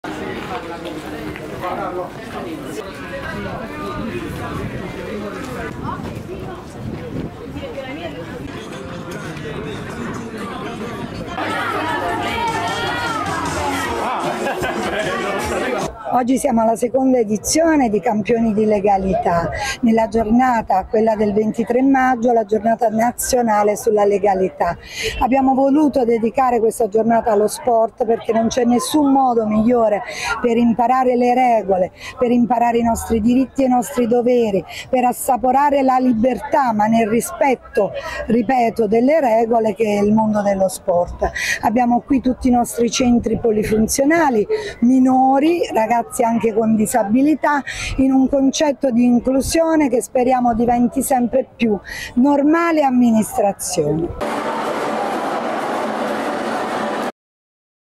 "Sport e Legalità": minori dei centri polifunzionali protagonisti al Palatulimieri e al Campo Volpe
Salerno:  minori dei centri polifunzionali protagonisti al Palatulimieri e al Campo Volpe col pregetto "Sport e Legalità"